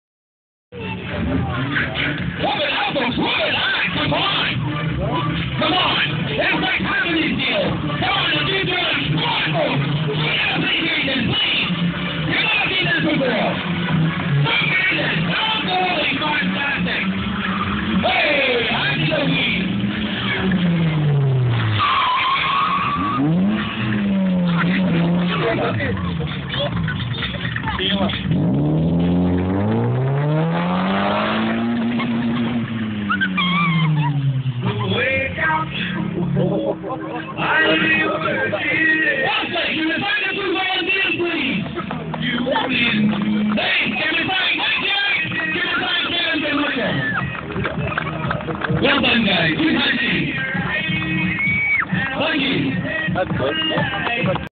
Motorbike show 2011
Stunts at the Scottish motorbike show Edinburgh